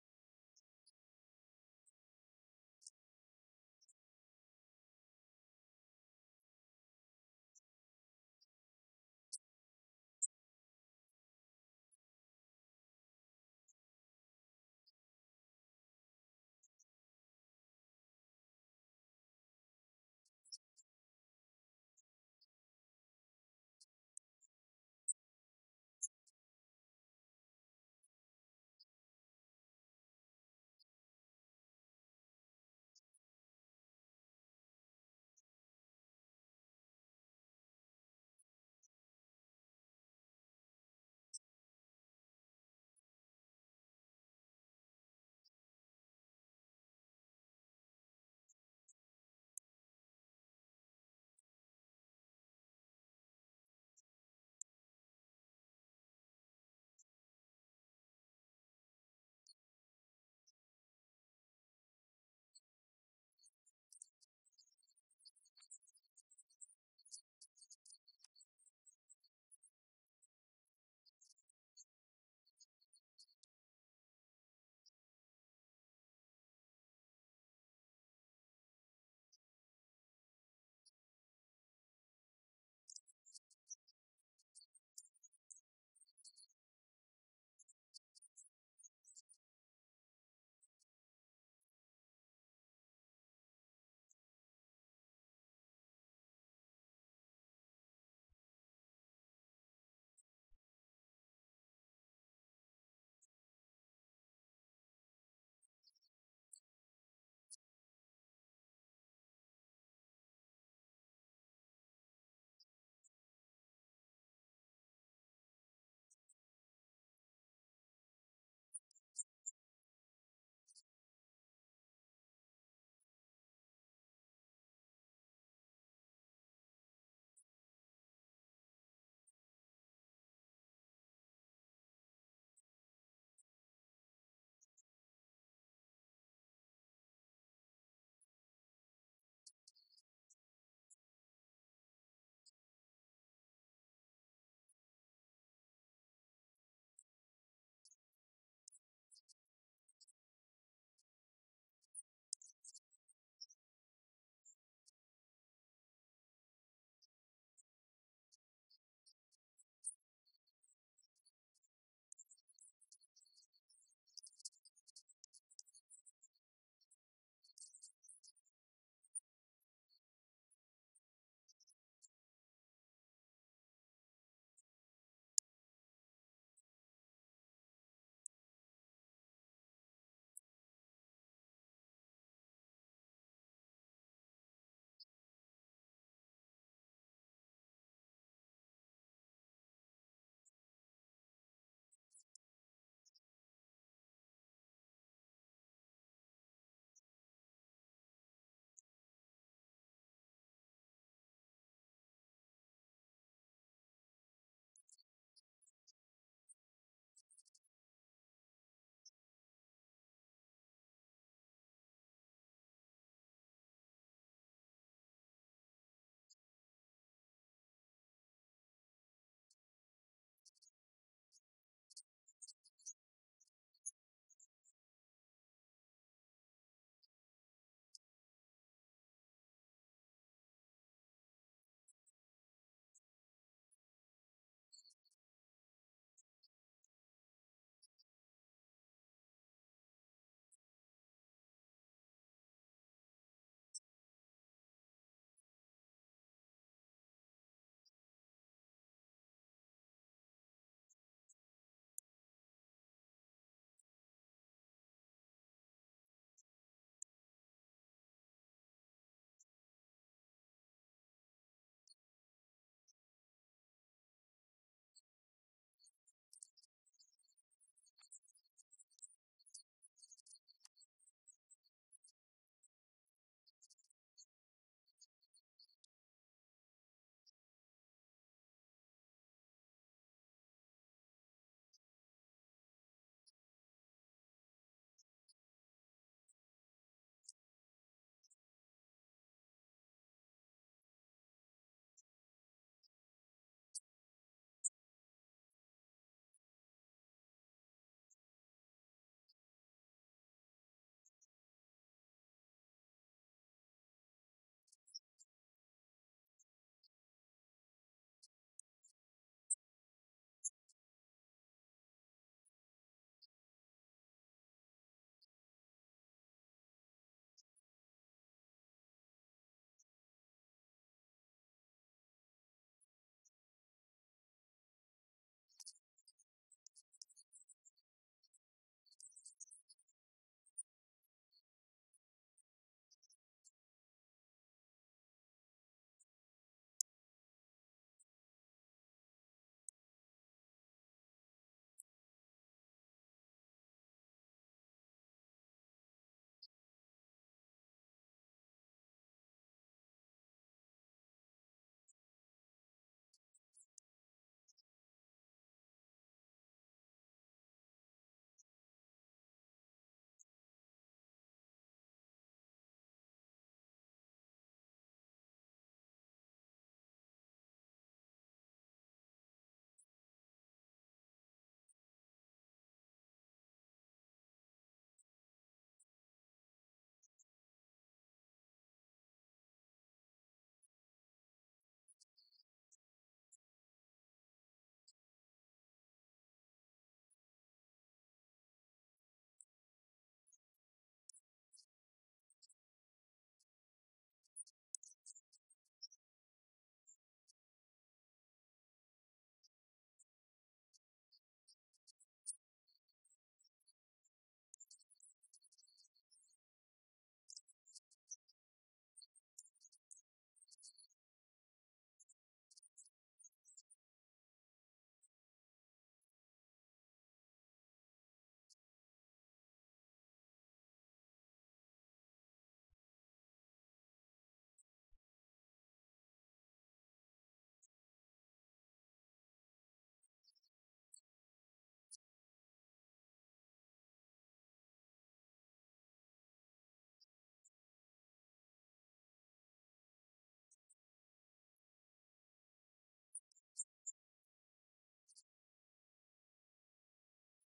Michael Angelo Mouse - children's story &amp; Chico Marx salute, too
Michael Angelo Mouse was written in 1945 by Katherine Evans. And for Children of Coincidence and Chico Marx, the voice for the narration will be kinda